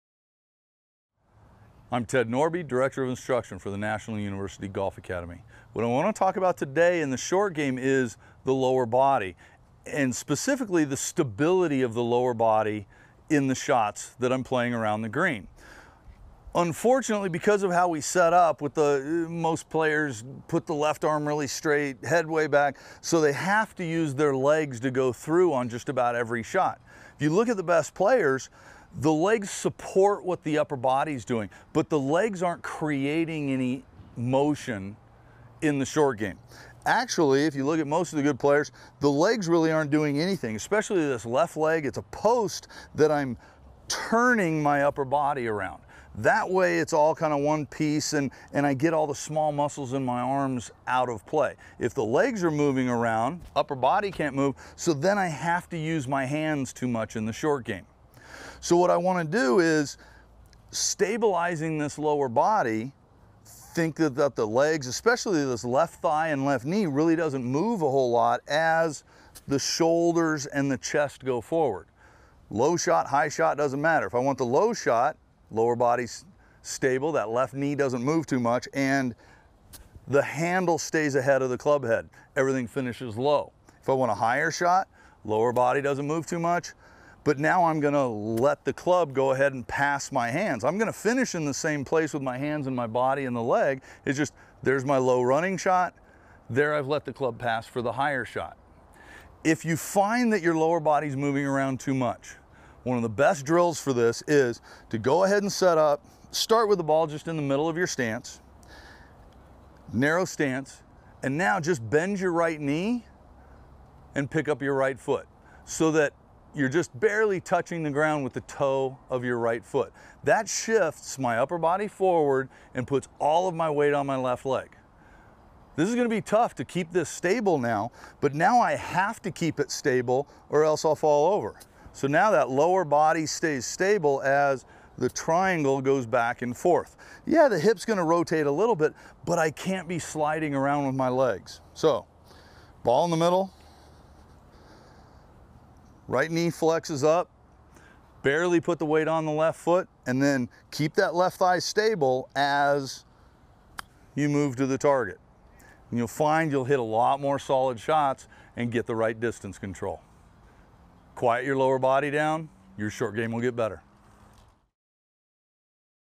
Golf Short Game A Stable Lower Body is Needed in the Short Game
today to learn more about training for a career in golf at National University Golf Academy.

AUDIO FROM THE VIDEO
I'm Ted Norby Director of Instruction for the National University Golf Academy.  What I want to talk about today in the short game is the lower body and specifically the stability of the lower body in the shots that I'm playing around the green.  Unfortunately because of how we set up, with the most players put their left arm really straight, head way back so they have to use their legs to go through on just about every shot.  You look at the best players the legs support what the upper body's doing.  But the legs aren't creating any motion, in the short game.  Actually if you look at most of the good players, the legs really aren't doing anything especially this left leg, it's a post that I'm turning my upper body around.  That way it's all kind of one piece and, and I get all the small muscles in my arms out of play.  If the legs are moving around, upper body can't move so then I have to use my hands to much in the short game.

So what I want to do is, stabilizing this lower body, think about the legs especially this left thigh and left knee really doesn't move a whole lot as the shoulders and the chest go forward.  Low shot, high shot, doesn't matter.  If I want the low shot, lower body's stable, that left knee doesn't move too much, and the handle stays ahead of the club head.   Everything finishes low.  If I want a higher shot, lower body doesn't move too much but now I'm gonna let the club go ahead and pass my hands.  I'm gonna finish in the same place with my hands and my body and the leg, it's just there's my low running shot, there I've let the club pass for the higher shot.

 If you find that your lower body's moving around too much, one of the best drills for this is to go ahead and set up, start with the ball just in the middle of your stance, narrow stance and now just bend your right knee and pick up your right foot.  So that your just barely touching the ground with the toe of your right foot.  That shifts my upper body forward and puts all of my weight on my left leg.  This is going to be tough to keep this stable now but now I have to keep it stable or else I'll fall over.  So now that lower body stays stable, as the triangle goes back and forth.   Yea, the hips gonna rotate a little bit but I can't be sliding around with my legs.  So, ball in the middle, right knee flexes up, barely put the weight on the left foot and then keep that left thigh stable as you move to the target.  And you'll find you'll hit a lot more solid shots and get the right distance control.  Quiet your lower body down; your short game will get better.